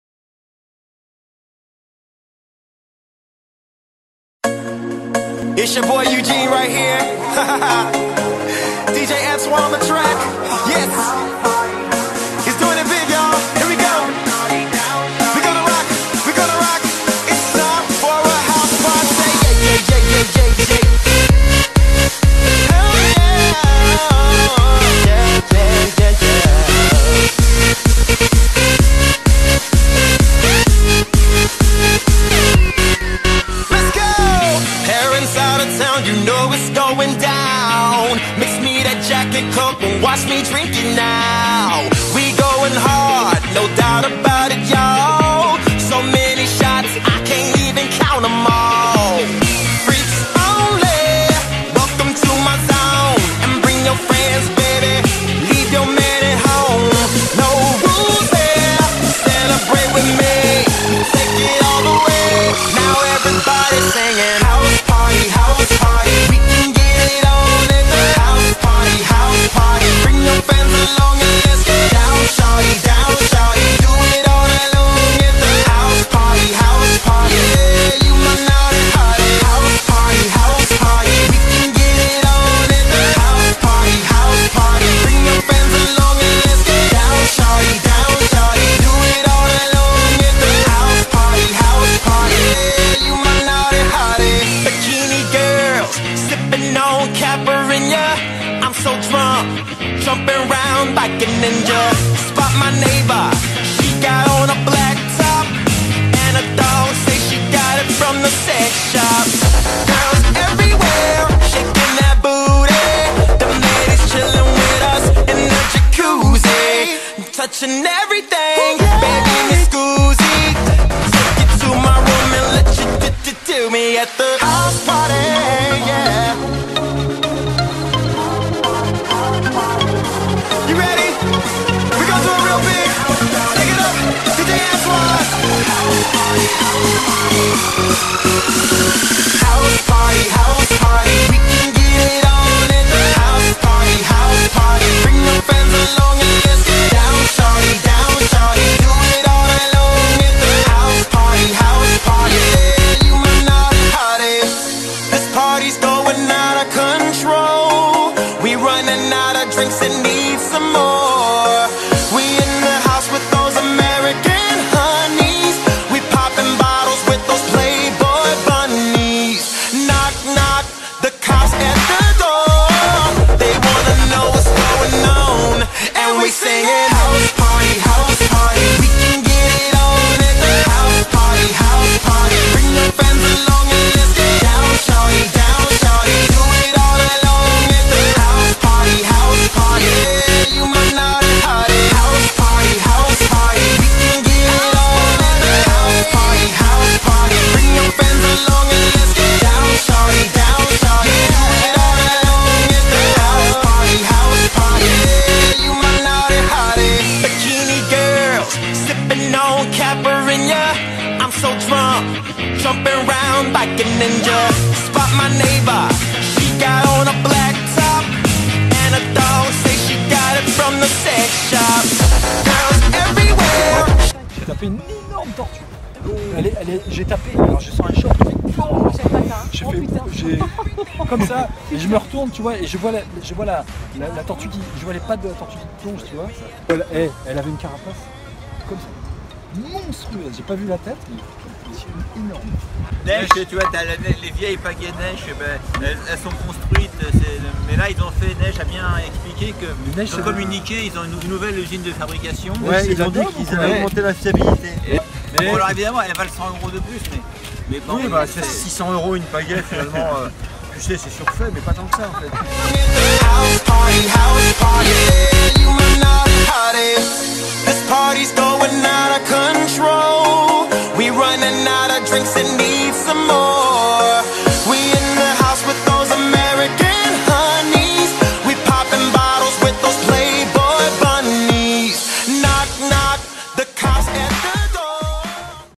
Stand Up Paddle Cimetière de St Leu - St Gilles
Un gros Down wind en SUP entres copains du spot du Cimetière de Saint Leu à Saint Gilles les bains. 18Km dans une grosse houle avec 70 à 80 km/h de vent dans le dos. Un pur moment de glisse !